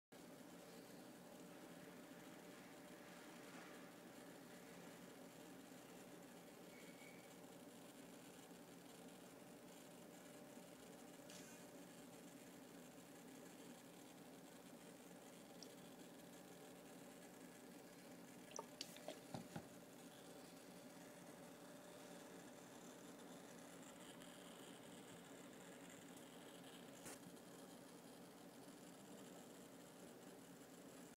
Rust-oleum Neverwet Demo closeup water on receiver chip board
Waterboarding my scx10 receiver while it is on!